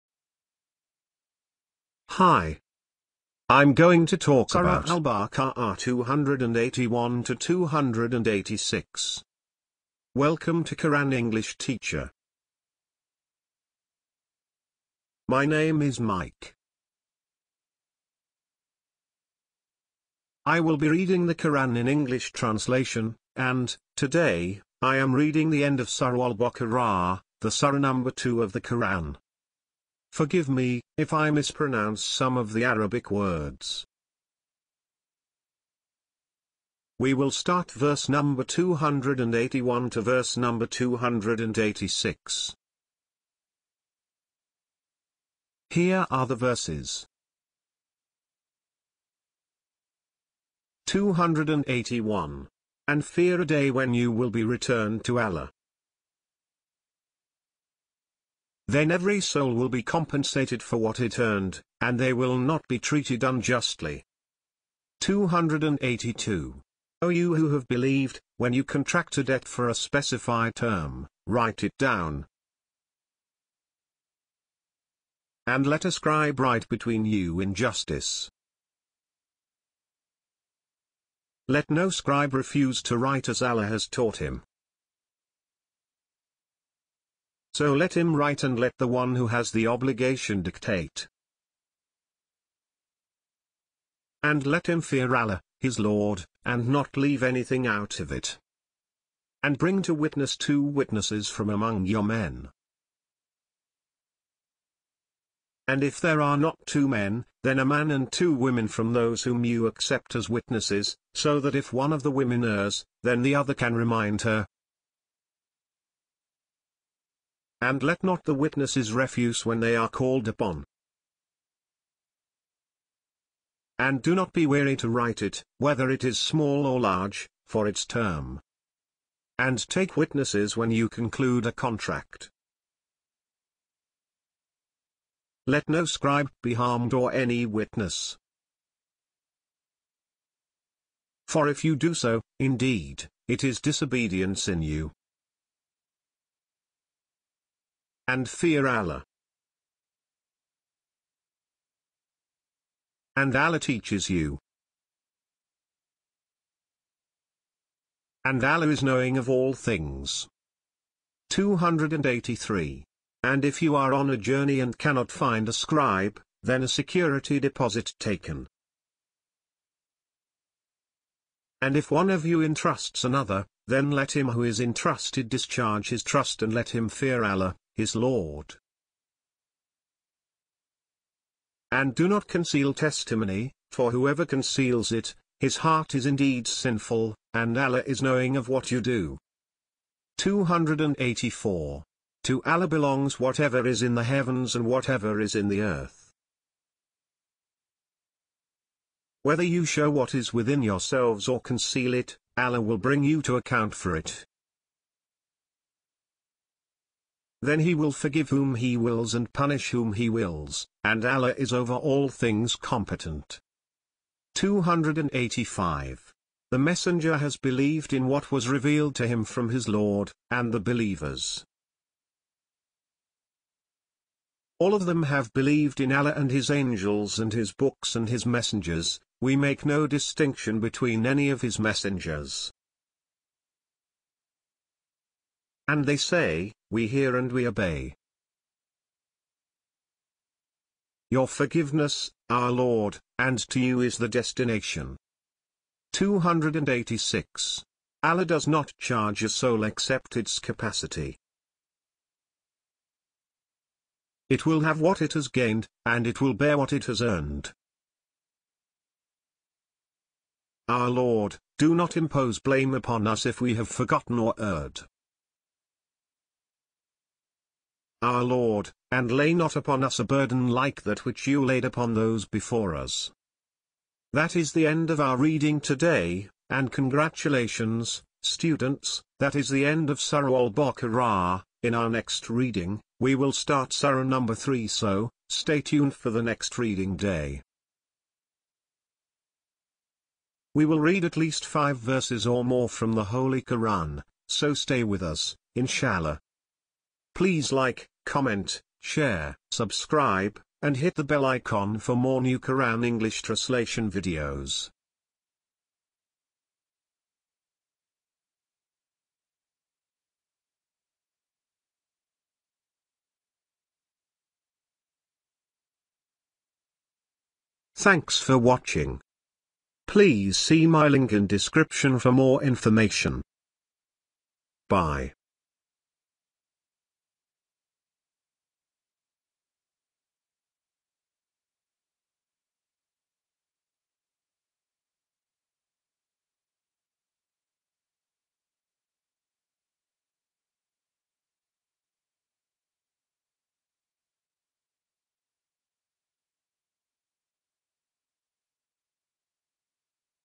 Quran English Teacher | Surah Al Baqarah 281 286
Welcome to Quran English Teacher
Today, we are counting, to read, Surah Al-Baqarah, it is the Longest Surah in the Quran, it has 282 Verses and will be reading them in English Translation of the Quran,
Today, We will be reading Surah Al-Baqarah Verses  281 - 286
We will read five or more verses per day, so stay with us

quran englsh english quran quran english translation english quran translation # quran english translation audio quran audio